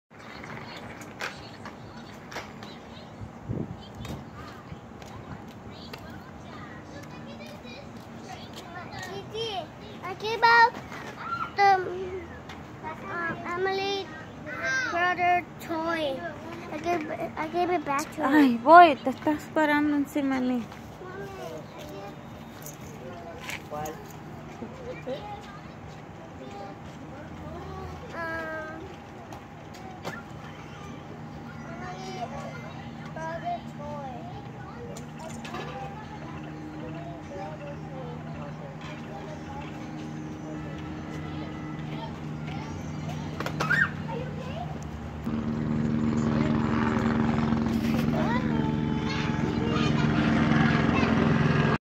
Put your bunny on a leash
Walking, hopping out side with the bunny